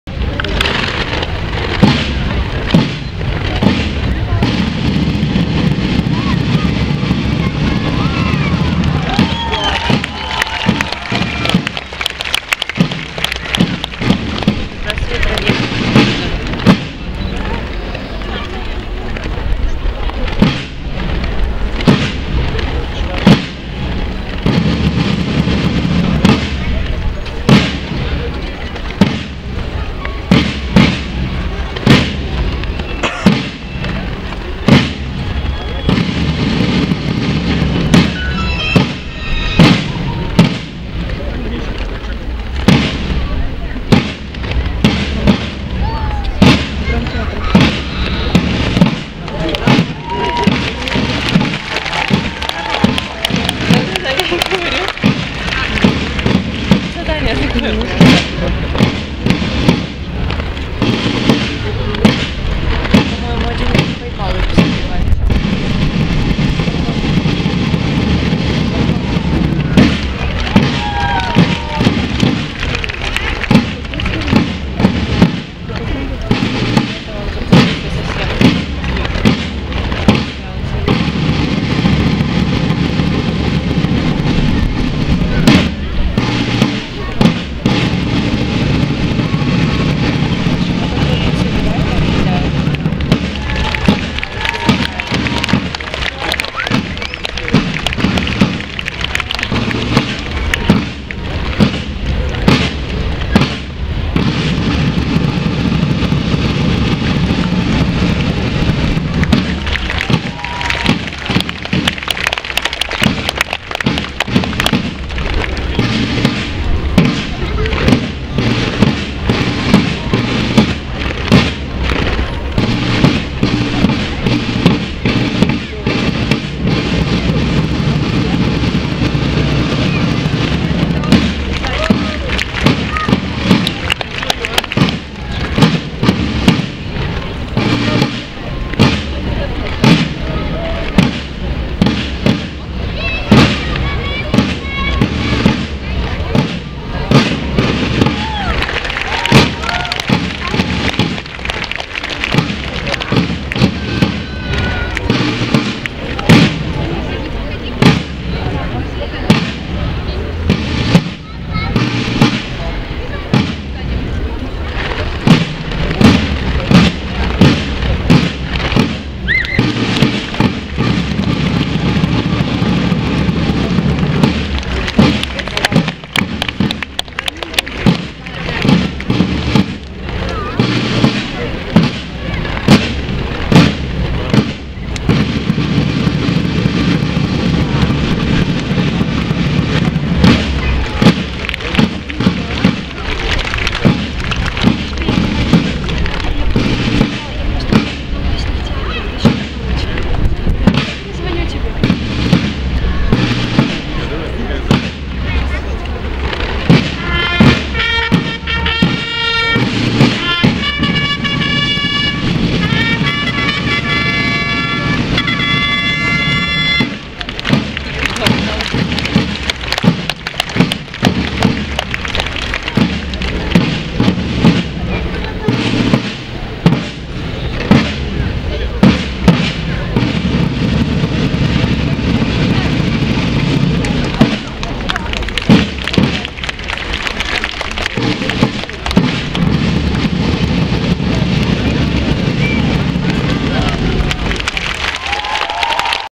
Флагоносцы Архангельск 2013 Team Performance flagonostsev of Arezzo (Italy)
Выступление команды флагоносцев из Ареззо (Италия). Фестиваль уличных театров. 27 июня 2013 город Архангельск. начало 20.10
Team Performance flagonostsev of Arezzo (Italy). Street Theatre Festival. June 27, 2013 the city of Arkhangelsk. start 20.10